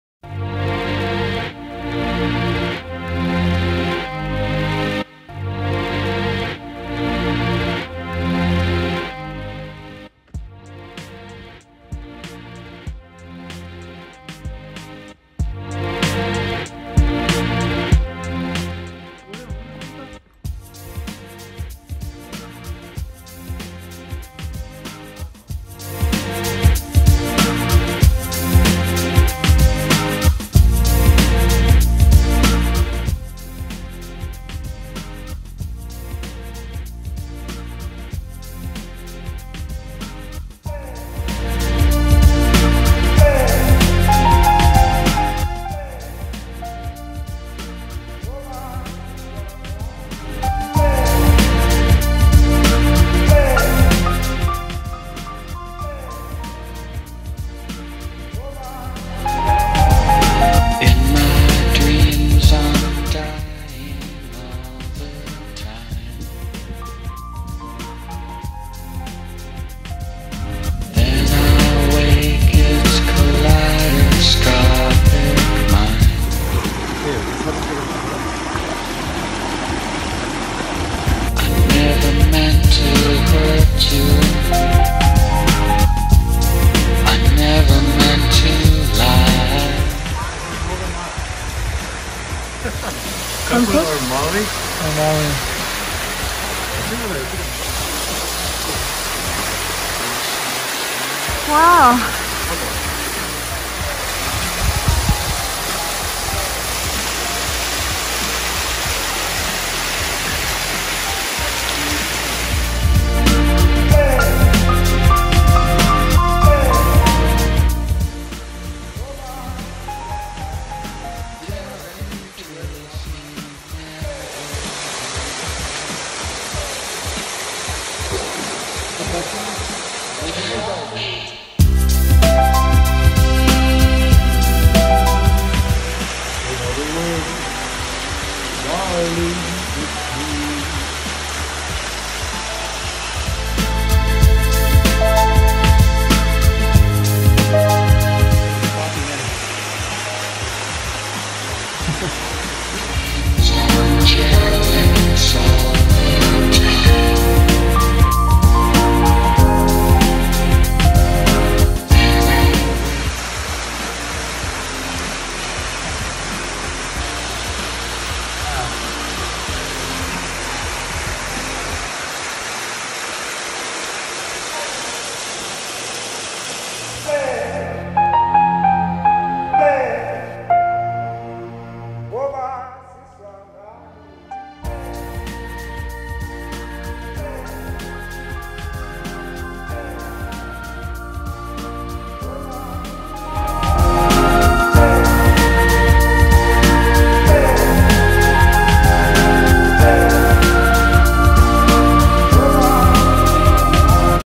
Hiking @ Angeles National forest, Buckhart trail, Los Angeles, California 7/2020
Hiking video of Buckhart trail@Angeles National Forest in Los Angeles, California#hiking#angelesnationalforest#buckharttrail#coopercanyon